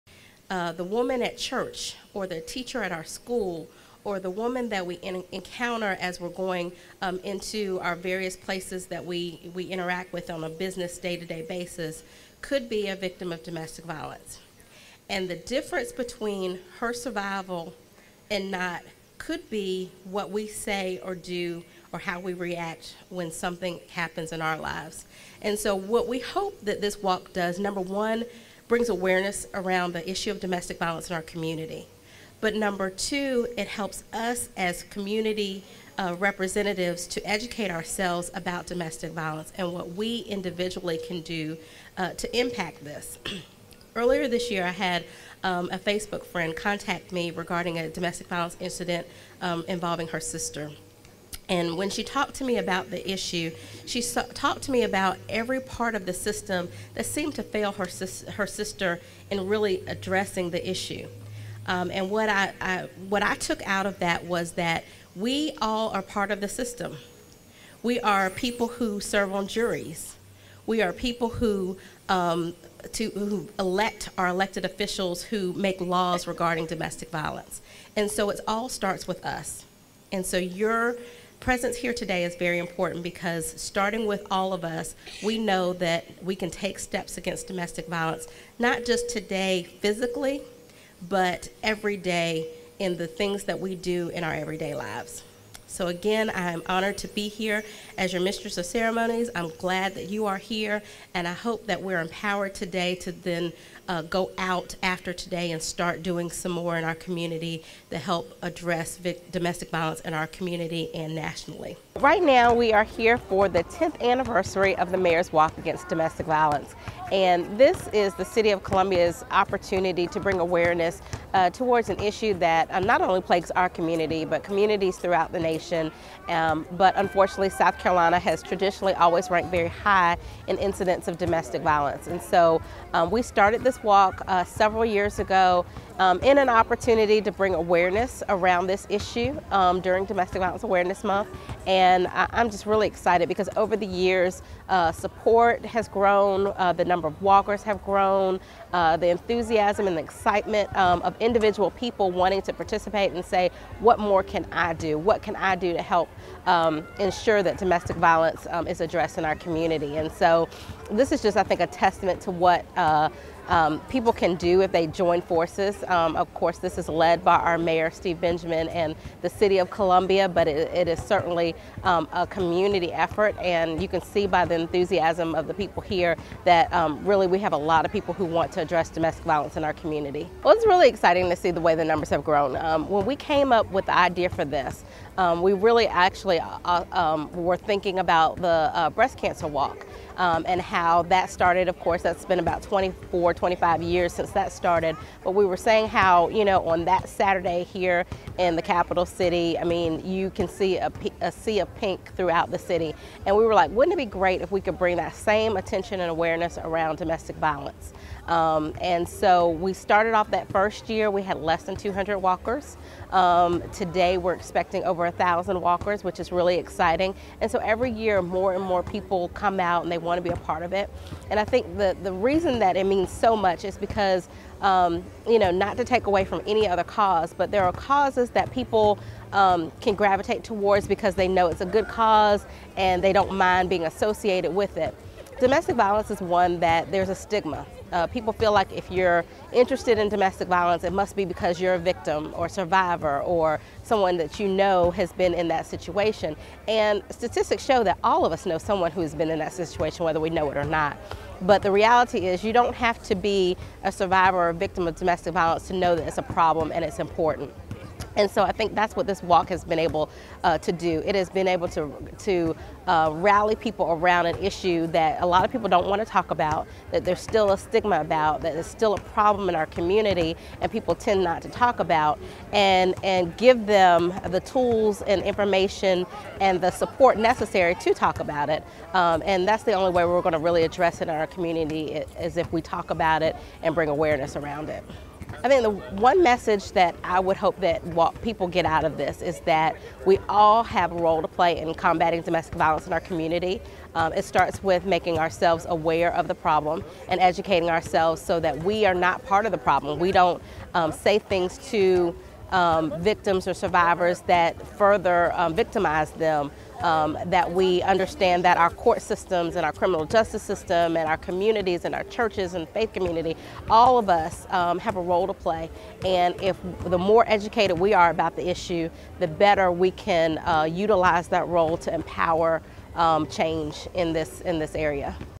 Tameika Isaac Devine opens up at the Domestic Violence Walk/Rally
South Carolina consistently ranks in the Top 10 of states for incidence of domestic violence.  Because of that, the City of Columbia started the Mayor's Walk Against Domestic Violence to bring awareness to this issue.

Tameika Isaac Devine opens up at the Domestic Violence Walk/Rally in Columbia, SC.  She explains the purpose of the walk/rally and how we all have a role to play in combatting domestic violence in our community.